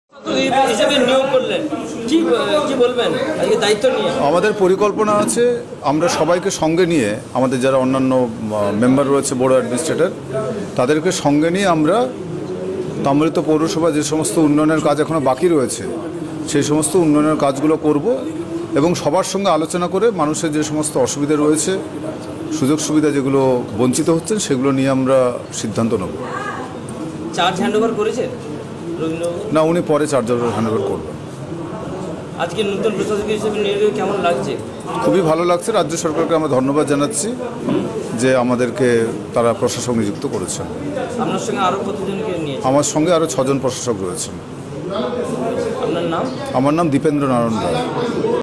কাঁথিরপর এবার তমলুক প্রশাসক রবীন্দ্রনাথ সেনকে সরানো হলো।
সোমবার রাজ্যের পুরো ও নগর উন্নয়ন দপ্তরের জয়েন্ট সেক্রেটারি এ নিয়ে একটি বিজ্ঞপ্তি জারি করেছেন।তাতে বলা হয়েছে পুরো বোর্ডের প্রশাসকের দায়িত্ব সামলাবে দীপেন্দ্র নারায়ন রায়। এছাড়াও প্রশাসক মন্ডলীতে সদস্য হিসেবে থাকবেন প্রাক্তন কাউন্সিলার কো-অর্ডিনেটর চন্দন প্রধান, সুব্রত রায়, শক্তিপদ ভট্টাচার্য, পৃথ্বীশ নন্দী, স্নিগ্ধা মিশ্র এবং চিত্ত মাইতি। ১১ নম্বর ওয়ার্ডের কো-অর্ডিনেটর বিশ্বজিৎ দত্ত বিজেপিতে যোগ দিয়েছেন। তার জায়গায় বিশিষ্ট আইনজীবী চিত্ত মাইতি কে প্রশাসক মন্ডলীতে রাখা হয়েছে।গত মে মাসে তমলুক, কাঁথি এবং এগরা পুরসভায় নির্বাচিত বোর্ড এর মেয়াদ শেষ হয়।তারপর এই তিন পুরসভায় চেয়ারম্যানদের পুরভোটের প্রশাসক এবং ভাইস চেয়ারম্যানদের বোর্ডের সদস্য হিসেবে নিযুক্ত করেছিল রাজ্য সরকার। দীপেন্দ্র নারায়ন রায় প্রাক্তন ভাইস চেয়ারম্যান এবং বোর্ডের সদস্য ছিলেন এবার তাকেই প্রশাসক হিসেবে নিযুক্ত করা হলো তমলুক পুরসভায়। কাঁথি পুরসভার পর তমলুক পুরসভায় প্রশাসকের রদবদল ঘটানো হলো।
বাইট-দীপের্ন্দ্র নারায়ন রায়, পৌর প্রসাশক , তমলুক